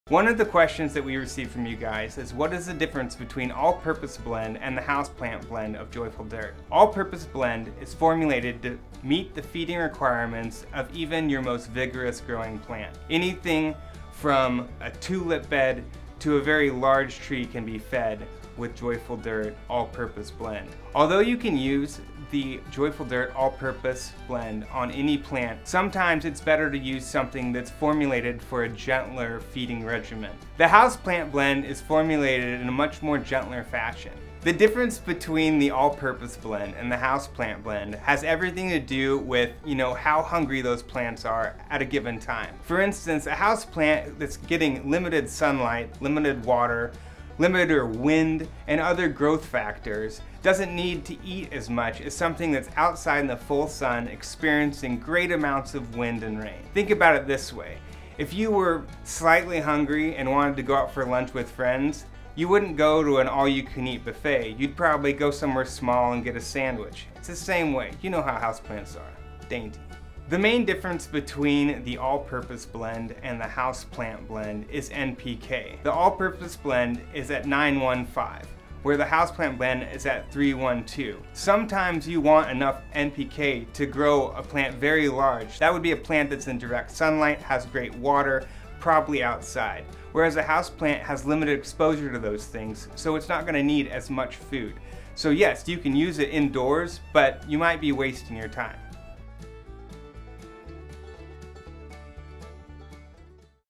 Joyful Dirt: Houseplant vs All-Purpose Blend
Drew from Joyful Dirt breaks down what the differences between the Houseplant blend and the All-Purpose blends.

The All-Purpose blend is designed to meet the requirements of variety of plants. You could use it on a tulip bed or even a large tree! You can use it on any plant, but there may be a better blend for the job depending on the plant.

The Houseplant blend is formulated in a much gentler fashion. The difference between the two has everything to do with how hungry those plants are. A houseplant will get more limited sunlight, wind, and rain events and therefore doesn't need to eat as much as something that's outside in the sun.

NPK is what separates the two. The All-Purpose blend is at 915, while the Housplant is at 312.